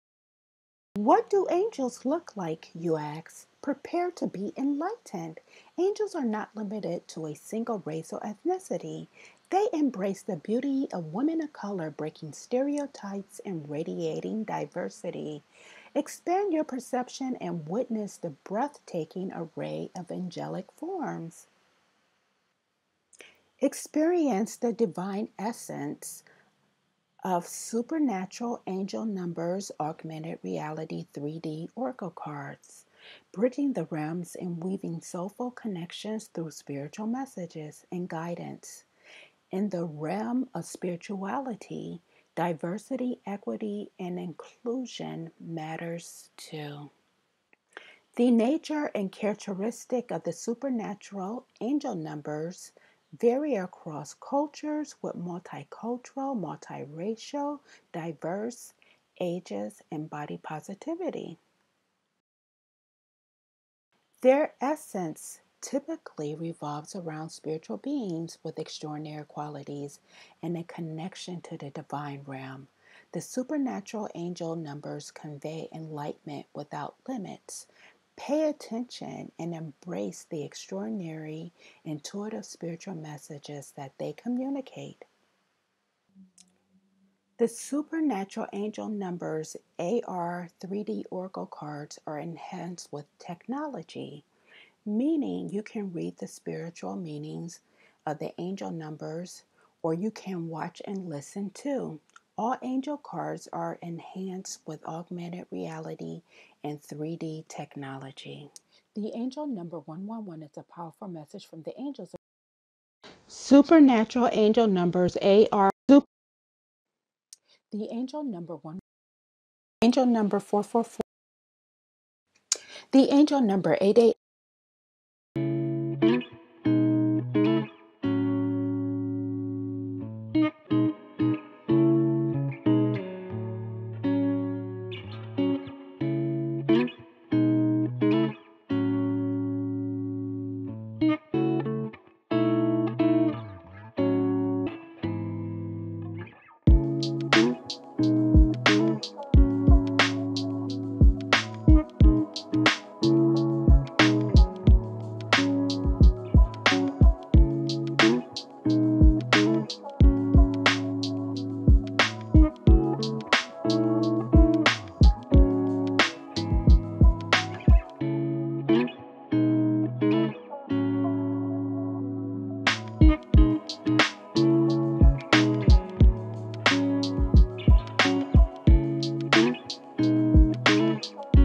SuperNatural Angel Numbers AR/3D Oracle Cards Numbers
Check out SuperNatural Angel Number AR & 3D Oracle Cards Demo.  Angel Numbers are known to have spiritual messages while giving guidance and strengthening your intuition.

What do angels look like, you ask? Prepare to be enlightened! Angels are not limited to a single race or ethnicity. They embrace the beauty of women of color, breaking stereotypes and radiating diversity. Expand your perception and witness the breathtaking array of angelic forms.

Experience the Divine Essence of our SuperNatural Angel Numbers AR/3D Oracle Cards, bridging realms and weaving soulful connections through spiritual messages. In the realm of spirituality, diversity, equity & inclusion  matters too.

The nature and characteristics of the SuperNatural Angel   Numbers vary across cultures with Multi-Cultural, Mult-Racial, Diverse Ages, and Body Positivity.

Their essence typically revolves around spiritual beings with extraordinary qualities and a connection to the divine realm.